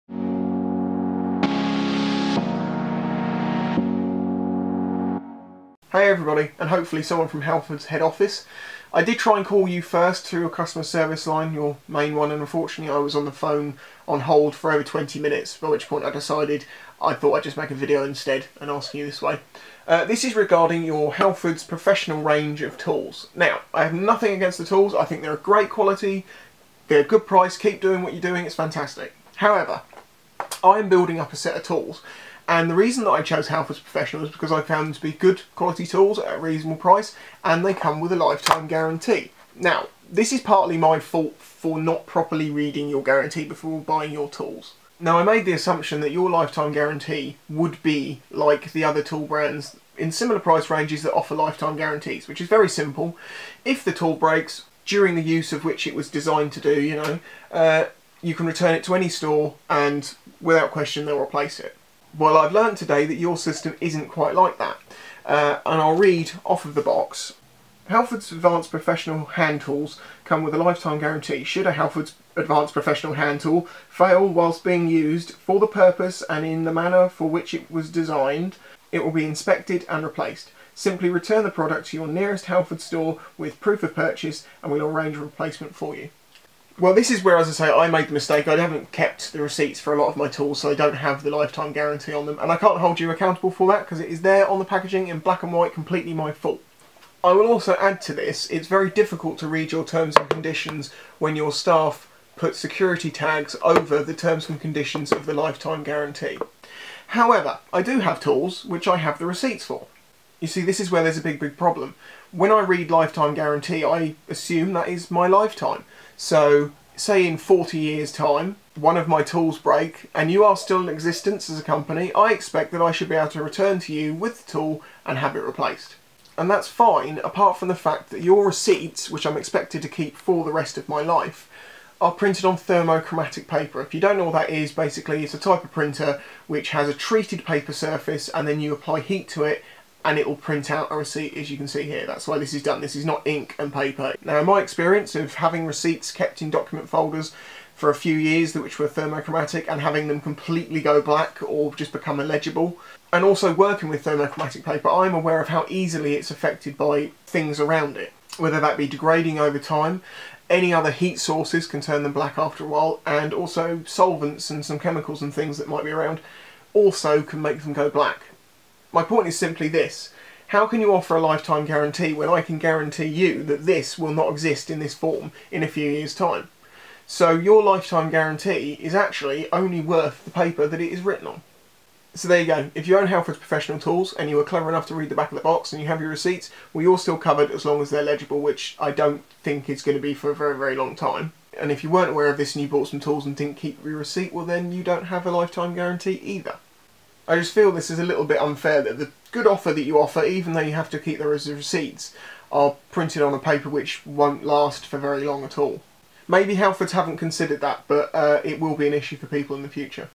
Dear Halfords....(Read Description)
please note the date of this video, These issue has been resolved! They were in fact changed only a very short while after the video came out a matter of a couple of weeks. coincidence? who knows lol

Here is more info from the comments if this is correct! Good work Halfords!

"As a halfords employee I can now let you know our terms and conditions have changed all halfords advanced tools are replace in accordance to the conditions on the back excluding the requirement for original receipt, this also includes the moving parts on ratchets swivel head adapters and breaker bars. The only point to add is occasionally we will have to order the replacement but at no extra cost to the customer other than returning in 2-3 working days to collect. Apologies on behalf of the company for the packaging not being changed but if you could add this information to the video we would be very happy. As a company we are aiming to reach all our customers needs and that is why the terms have changed. So in summary the receipts are not required and any halfords store will be happy to assist you in finding a suitable replacement part, thankyou

My thoughts expressed here are my own and do not represent any of the other parties associated with me."

The code will not work on items already under a offer.

Bike: Sinnis Apache 125cc with D.E.P 3 exhaust.
Helmet: Icon Variant .